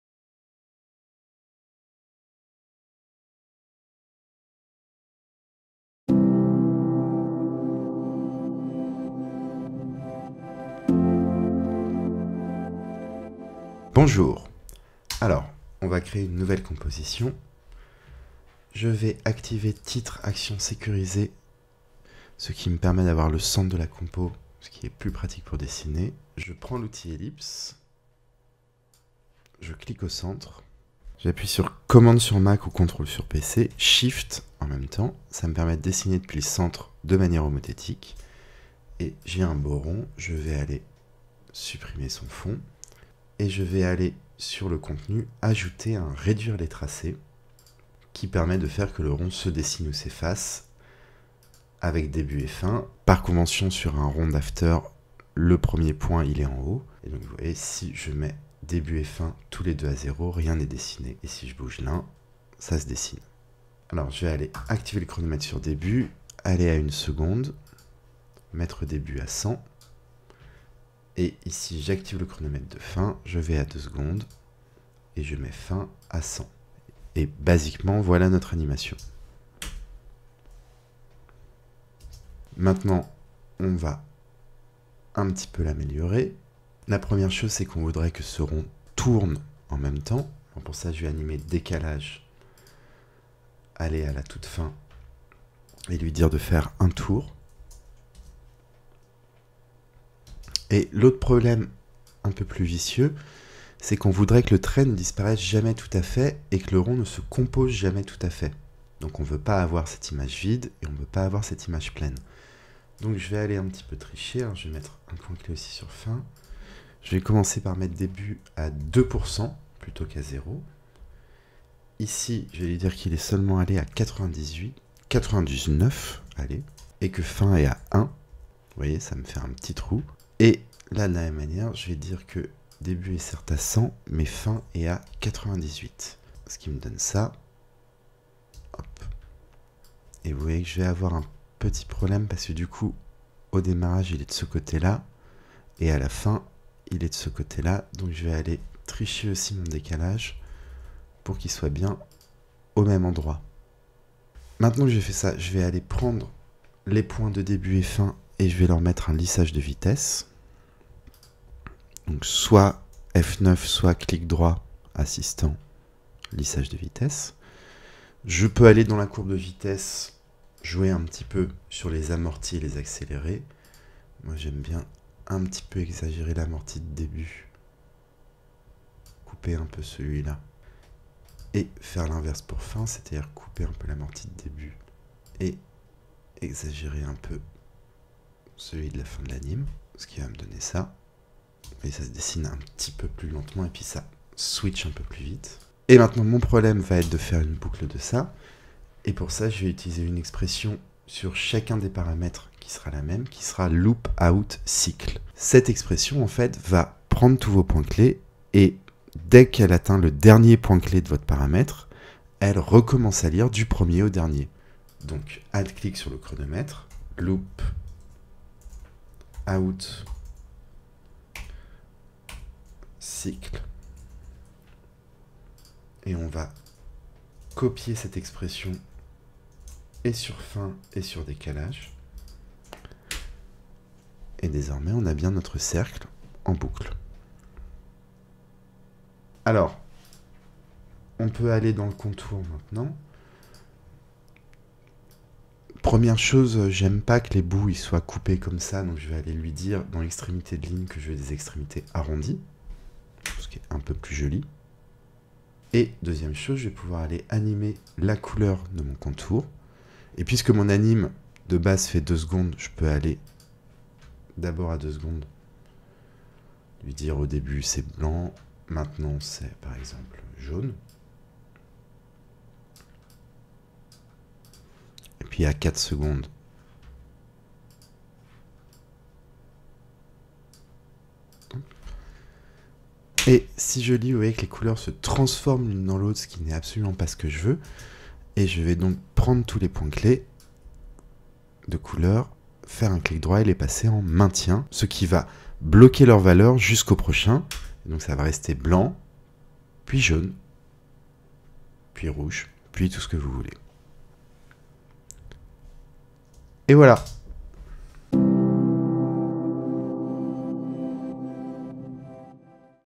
TIP #3. Créer un cercle de chargement en boucle.
Tutoriel pour créer un cercle de chargement à la google.

Expression à coller sur les trois paramètres du réduire les tracés :
loopOut("cycle")